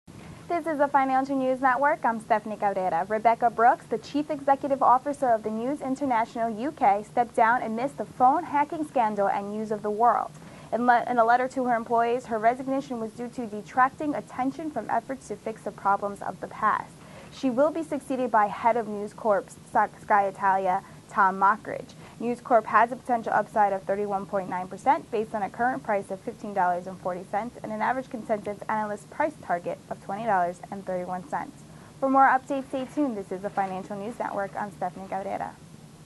News Corp.'s News International U.K. CEO Steps Down
Rebekah Brooks, the chief executive officer of the News International U.K. stepped down amidst the phone-hacking scandal at News of the World. In letter to her employees her resignation was due to "detracting attention" from efforts to "fix the problems of the past".

She will be succeeded by head of News Corp's Sky Italia Tom Mockridge.

News Corp. has a potential upside of 31.9% based on a current price of $15.4 and an average consensus analyst price target of $20.31.